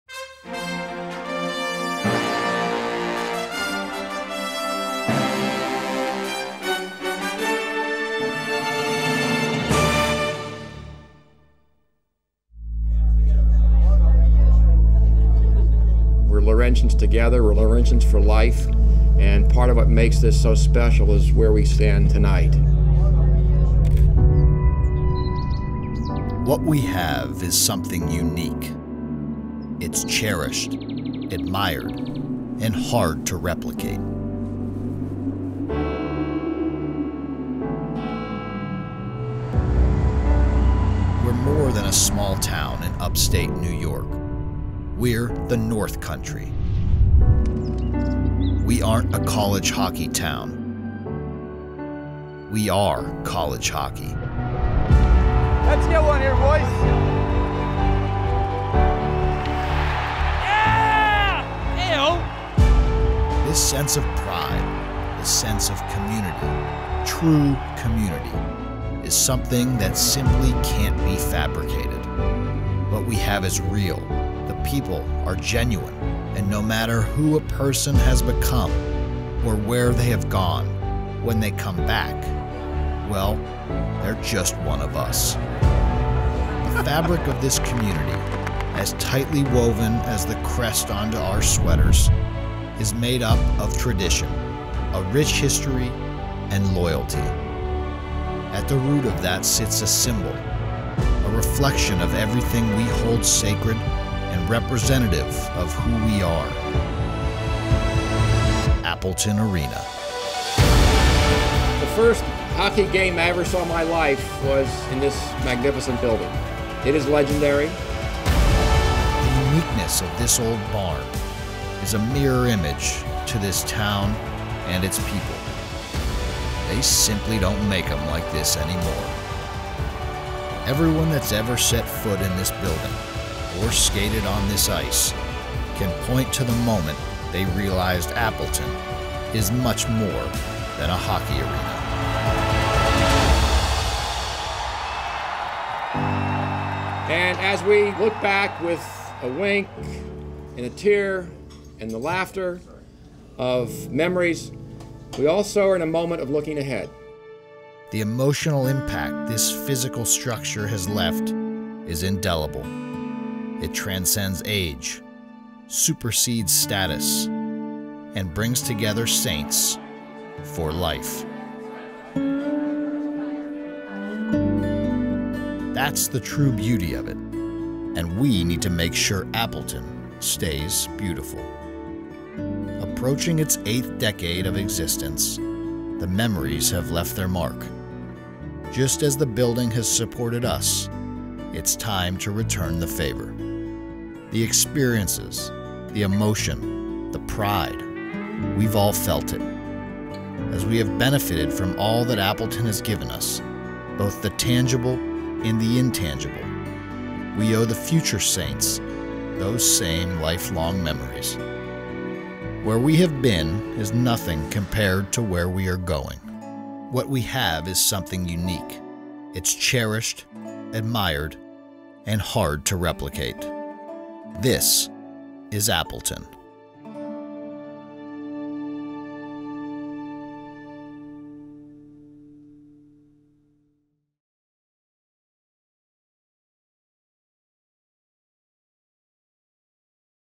The Tradition of Appleton Continues
Appleton is more than a hockey arena; it’s a home where Saints come together to pursue greatness. Now is your chance to be part of the team behind renovating one of the most beloved college hockey barns in North America. Learn how